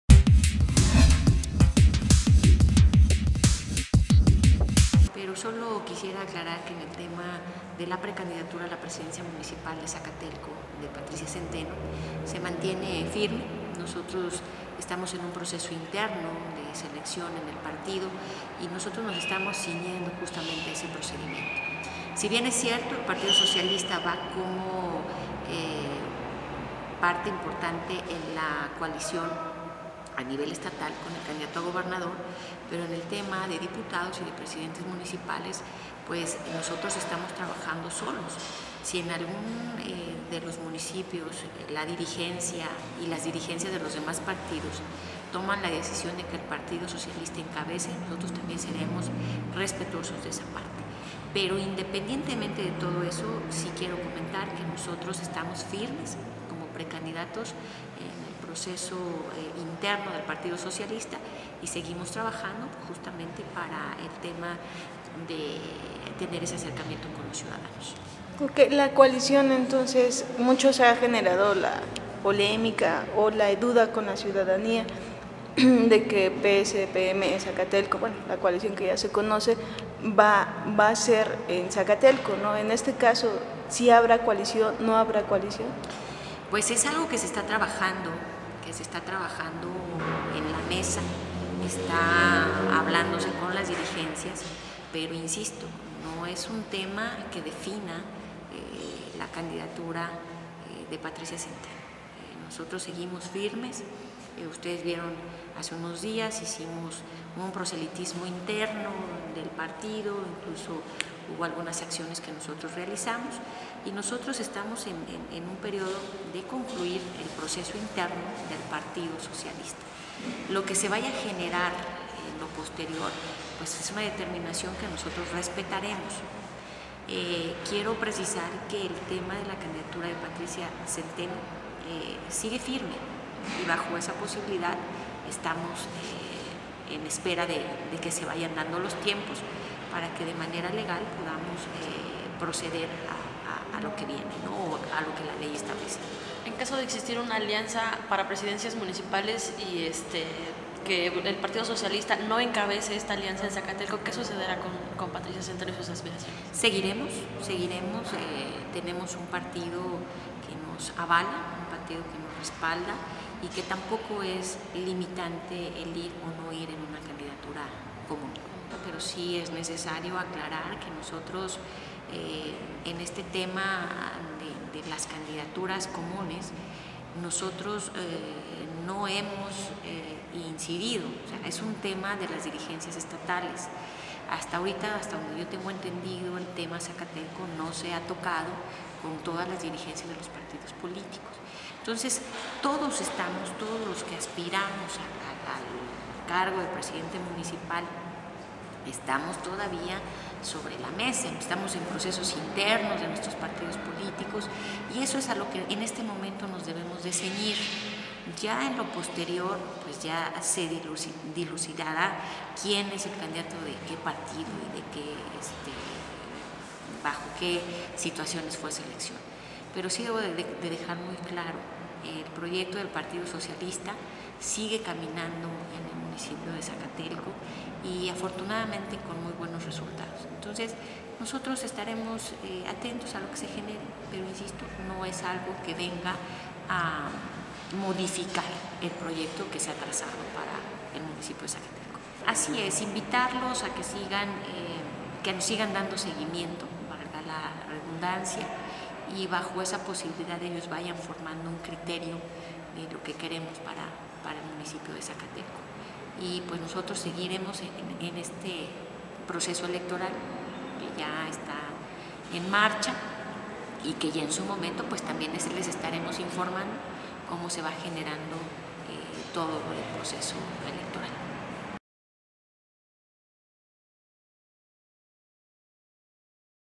Diputada Patricia Zenteno declara respecto a la coalición
La diputada Patricia Zenteno aclara el tema respecto a la colación que existe entre los partido PRI, PANAL, PS y PVEM y asimismo expone su postura al respecto